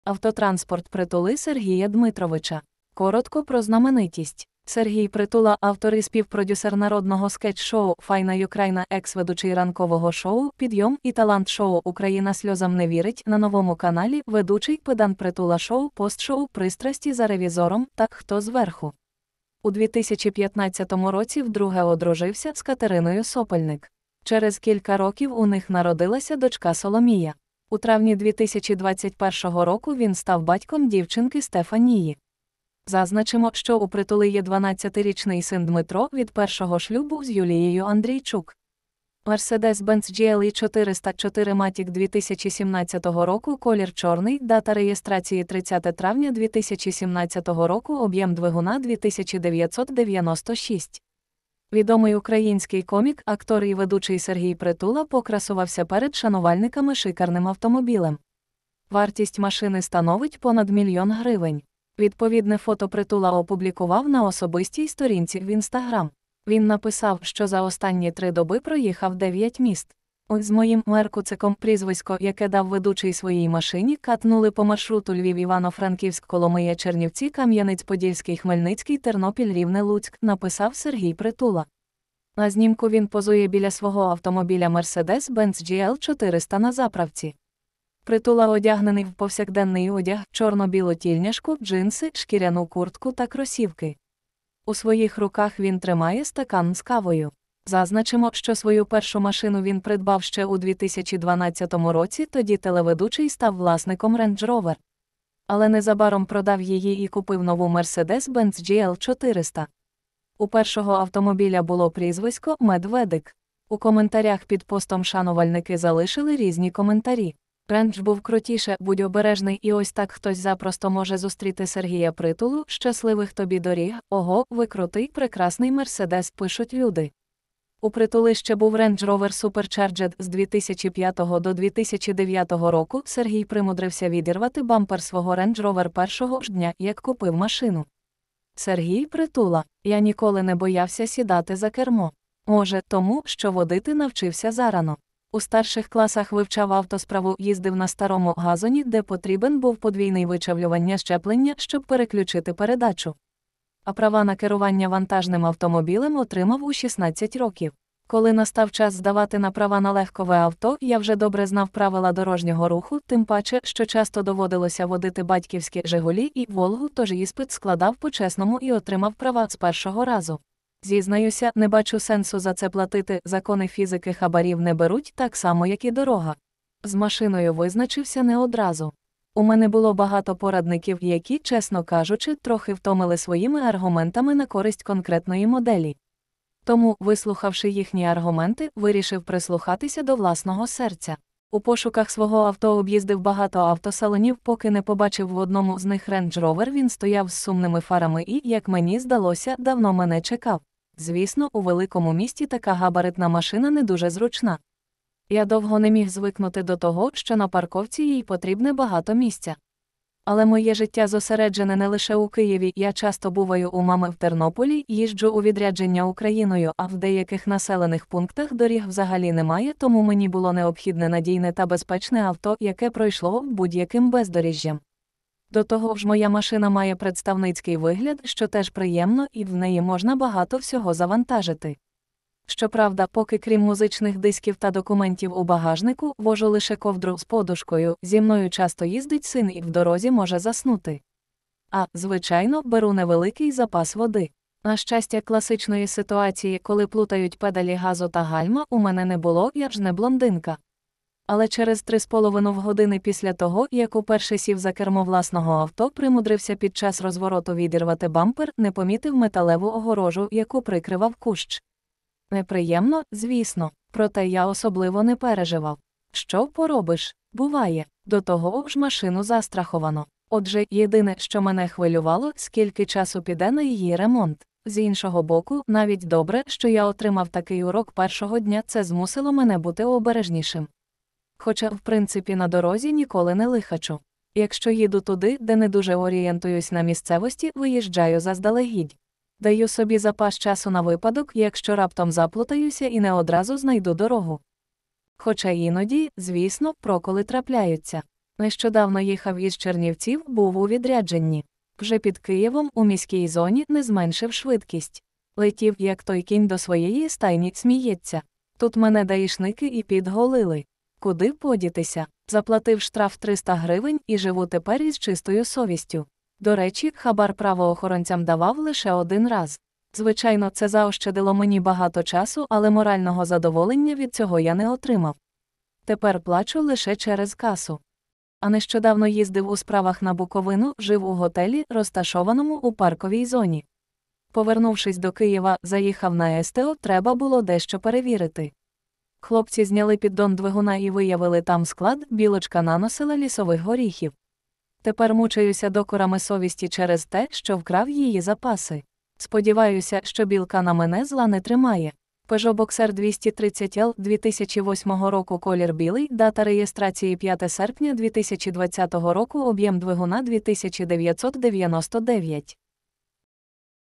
Автотранспорт Притули Сергія Дмитровича
Коротко про знаменитість:

• Сергій Притула – автор і співпродюсер народного скетч-шоу "Файна Юкрайна", ексведучий ранкового шоу "Підйом" і талант-шоу "Україна сльозам не вірить" на Новому каналі, ведучий "Педан-Притула шоу", постшоу "Пристрасті за Ревізором" та "Хто зверху?".

• У 2015 році вдруге одружився з Катериною Сопельник. Через кілька років у них народилася дочка Соломія. У травні 2021 року він став батьком дівчинки Стефанії. Зазначимо, що у Притули є 12-річний син Дмитро від першого шлюбу з Юлією Андрійчук.

MERCEDES-BENZ GLE 400 4 MATIC 2017 Колір Чорний, Дата реєстрації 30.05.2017 Об'єм двигуна 2996.

Відомий український комік, актор і ведучий Сергій Притула покрасувався перед шанувальниками шикарним автомобілем. Вартість машини становить понад мільйон гривень.

Відповідне фото Притула опублікував на особистій сторінці в Instagram. Він написав, що за останні три доби проїхав дев'ять міст.

"Ми з моїм "меркуциком" (прізвисько, яке дав ведучий своїй машині, – Ред.) катнули по маршруту Львів-Івано-Франківськ-Коломия-Чернівці-Кам'янець-Подільський-Хмельницький-Тернопіль-Рівне-Луцьк", – написав Сергій Притула.

На знімку він позує біля свого автомобіля Mercedes-Benz GL 400 на заправці. Притула одягнений в повсякденний одяг: чорно-білу тільняшку, джинси, шкіряну куртку та кросівки. У своїх руках він тримає стакан з кавою.

Зазначимо, що свою першу машину він придбав ще у 2012 році – тоді телеведучий став власником Range Rover. Але незабаром продав її і купив нову – Mercedes-Benz GL 400. У першого автомобіля було прізвисько "Медведик".

У коментарях під постом шанувальники залишили різні коментарі.

"Рендж був крутіше", "Будь обережний", "І ось так хтось запросто може зустріти Сергія Притулу", "Щасливих тобі доріг", "Ого, ви крутий", "Прекрасний мерседес", – пишуть люди.

У Притули ще був Range Rover Supercharged (2005—2009), Сергій примудрився відірвати бампер свого Range Rover першого ж дня, як купив машину.

Сергій Притула : Я ніколи не боявся сідати за кермо. Може, тому, що водити навчився зарано. У старших класах вивчав автосправу - їздив на старому «газоні», де потрібен був подвійний вичавлювання зчеплення, щоб переключити передачу. А права на керування вантажним автомобілем отримав у 16 ​​років.

Коли настав час здавати на права на легкове авто, я вже добре знав правила дорожнього руху. Тим паче, що часто доводилося водити батьківські «Жигулі», і «Волгу». Тож іспит складав по-чесному і отримав права з першого разу. Зізнаюся, не бачу сенсу за це платити - закони фізики хабарів не беруть, так само як і дорога.

З машиною визначився не одразу. У мене було багато порадників, які, чесно кажучи, трохи втомили своїми аргументами на користь конкретної моделі. Тому, вислухавши їхні аргументи, вирішив прислухатися до власного серця. У пошуках свого авто об'їздив багато автосалонів, поки не побачив в одному з них Range Rover - він стояв з сумними фарами і, як мені здалося, давно мене чекав.

Звісно, ​​у великому місті така габаритна машина не дуже зручна. Я довго не міг звикнути до того, що на парковці їй потрібне багато місця. Але моє життя зосереджене не лише у Києві – я часто буваю у мами в Тернополі, їжджу у відрядження Україною. А в деяких населених пунктах доріг взагалі немає, тому мені було необхідне надійне та безпечне авто, яке пройшло б будь-яким бездоріжжям. До того ж моя машина має представницький вигляд, що теж приємно. І в неї можна багато всього завантажити. Щоправда, поки крім музичних дисків та документів у багажнику, вожу лише ковдру з подушкою: зі мною часто їздить син і в дорозі може заснути. А, звичайно, беру невеликий запас води.

На щастя, класичної ситуації, коли плутають педалі газу та гальма, у мене не було – я ж не блондинка! Але через 3,5 години після того, як уперше сів за кермо власного авто, примудрився під час розвороту відірвати бампер - не помітив металеву огорожу, яку прикривав кущ. Неприємно, звісно... Проте я особливо не переживав. Що поробиш? Буває! До того ж машину застраховано. Отже, єдине, що мене хвилювало, - скільки часу піде на її ремонт.

З іншого боку, навіть добре, що я отримав такий урок першого дня — це змусило мене бути обережнішим. Хоча в принципі на дорозі ніколи не лихачу. Якщо їду туди, де не дуже орієнтуюсь на місцевості, виїжджаю заздалегідь. Даю собі запас часу на випадок, якщо раптом заплутаюся і не одразу знайду дорогу.

Хоча іноді, звісно, ​​проколи трапляються. Нещодавно їхав із Чернівців – був у відрядженні. Вже під Києвом, у міській зоні, не зменшив швидкість. Летів, як той кінь до своєї стайні (сміється). Тут мене даішники і «підголили»… Куди подітися? Заплатив штраф 300 грн – і живу тепер із чистою совістю. До речі, хабар правоохоронцям давав лише один раз. Звичайно, це заощадило мені багато часу, але морального задоволення від цього я не отримав. Тепер плачу лише через касу.

А нещодавно їздив у справах на Буковину. Жив у готелі, розташованому у парковій зоні.